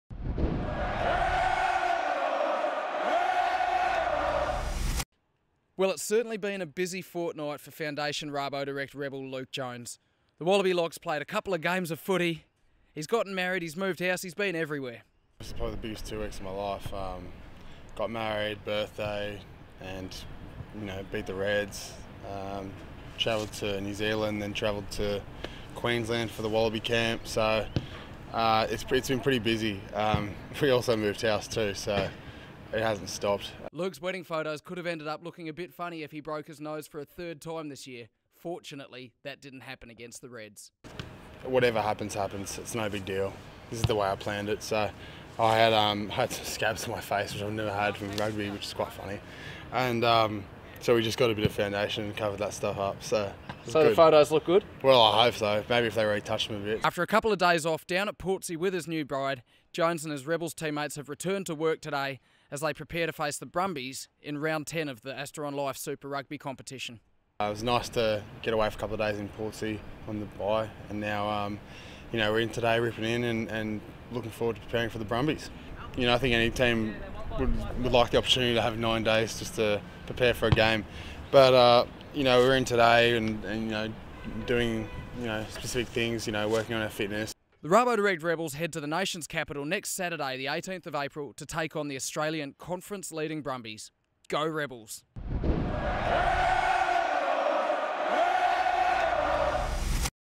REB TV: Luke Jones' busy fortnight
Foundation RaboDirect Rebel Luke Jones discusses the busiest fortnight of his life, while the Wallaby lock also looks ahead to the Round 10 Super Rugby clash with the Brumbies.